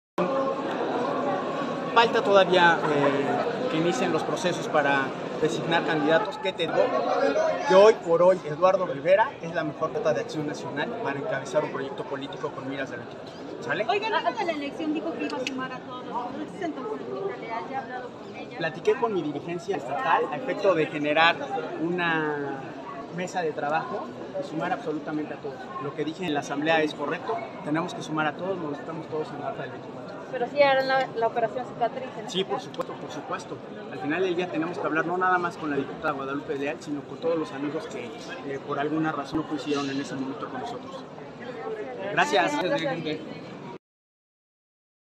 Jesús Saldivar afirma que hoy por hoy Eduardo Rivera es la mejor carta del PAN
Jesús Saldivar, dirigente municipal del PAN dijo que ''Hoy por hoy Eduardo Rivera es la mejor carta de Acción Nacional para encabezar un proyecto político" de cara a 2024